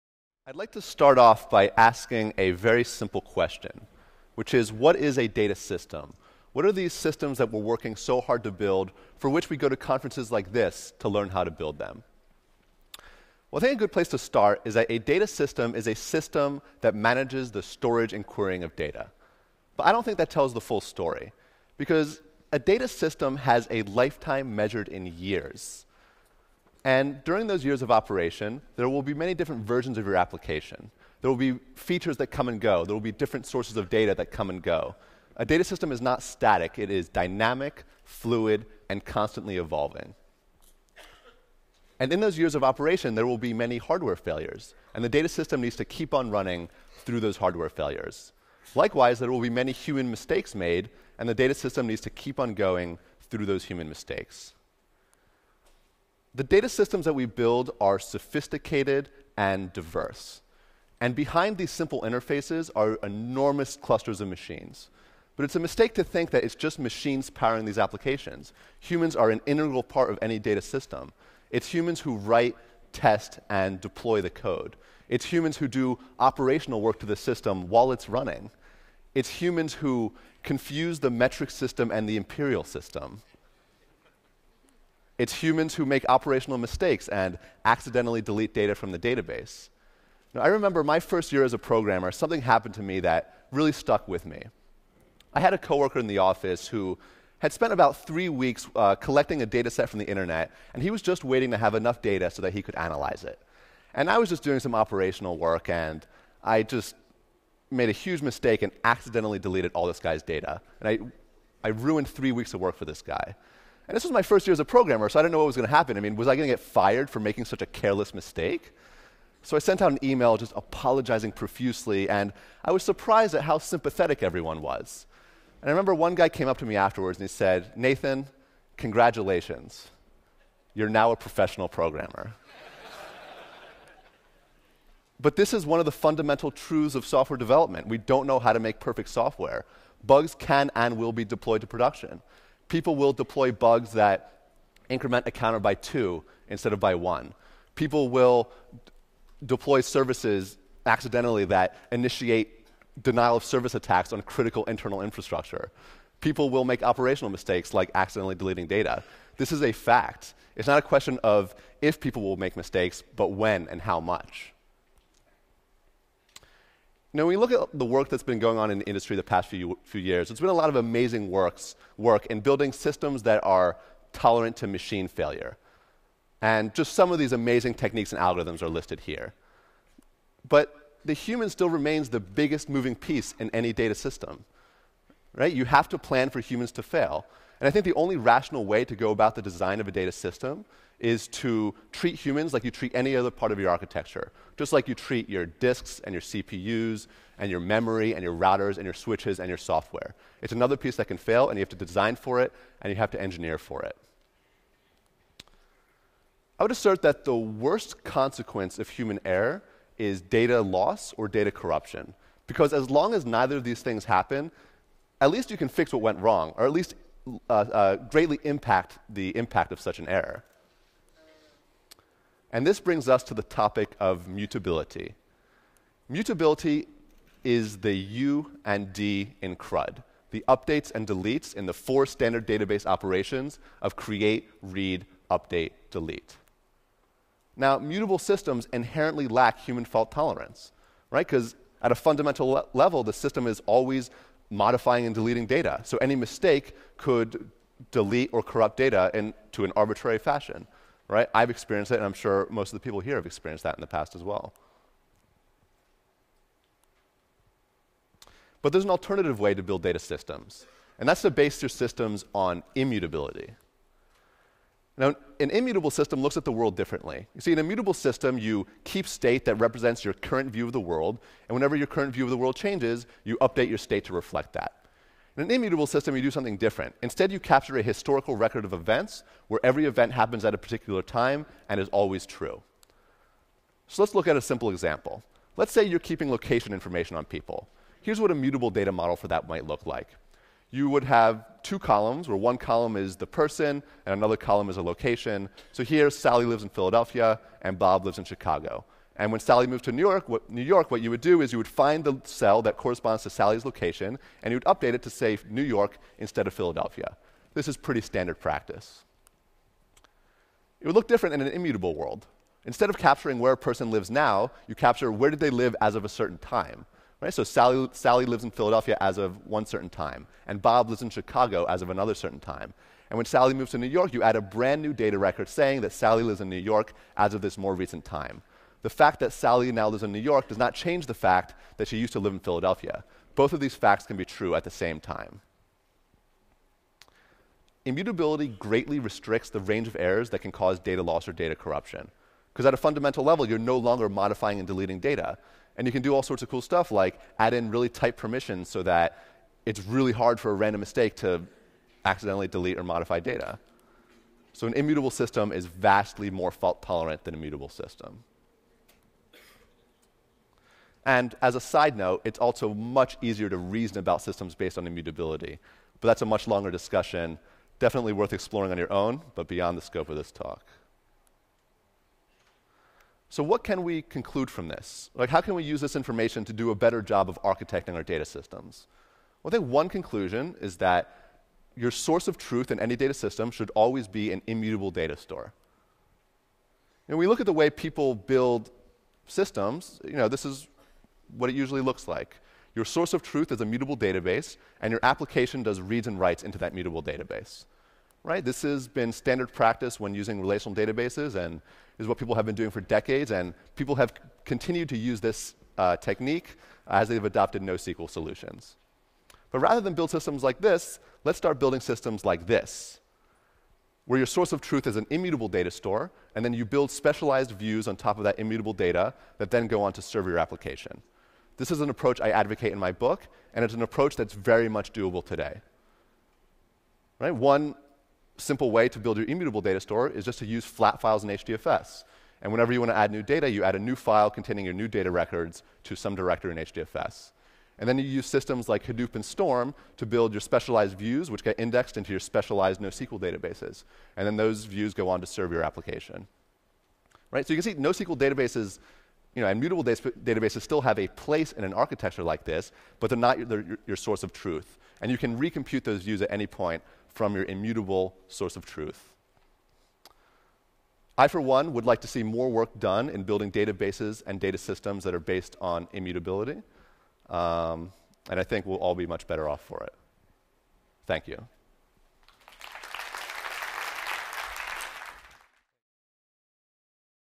Strata 2013: Nathan Marz, "Human Fault-tolerance"
There's been a huge amount of progress in recent years in developing distributed systems that are resilient to all sorts of faults. However, there's one critical category of errors that has largely been ignored: human error. The scope and potential impact of human error is massive: deployed bugs, accidentally deleting data, accidentally DDOS'ng important internal services, and so on. Designing for human fault-tolerance leads to important conclusions on the fundamental ways data systems should be architected.